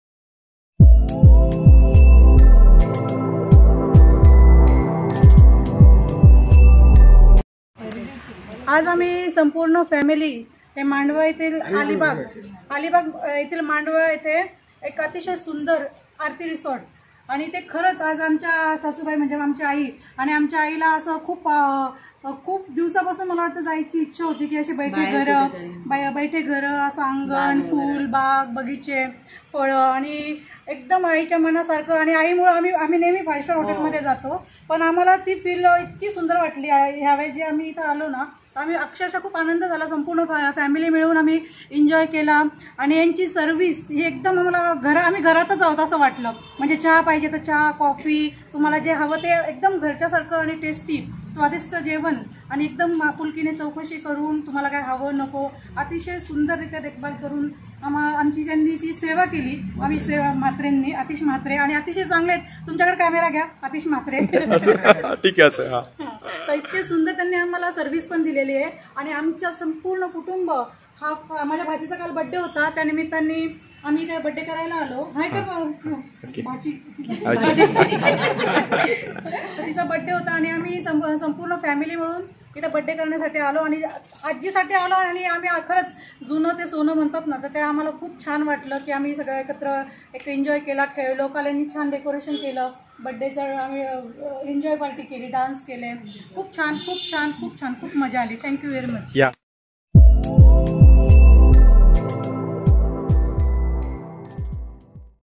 सौ. शिलाताई गांगुर्डे राष्ट्रीय चिटणीस श्री.अनिल गांगुर्डे, उपाध्यक्ष महाराष्ट्र RPI आरती रिसॉर्ट
आज १३.०३.२०२१ सौ. शिलाताई गांगुर्डे RPI राष्ट्रीय चिटणीस श्री. अनिल गांगुर्डे RPI उपाध्यक्ष महाराष्ट्र प्रदेश ह्यांनी संपूर्ण कुटुंबासमवेत आपल्या आरती रिसॉर्ट मांडवा अलिबाग येथे 2 दिवस केलेली धम्माल ह्या व्हिडीओ मध्ये पाहायला मिळेल.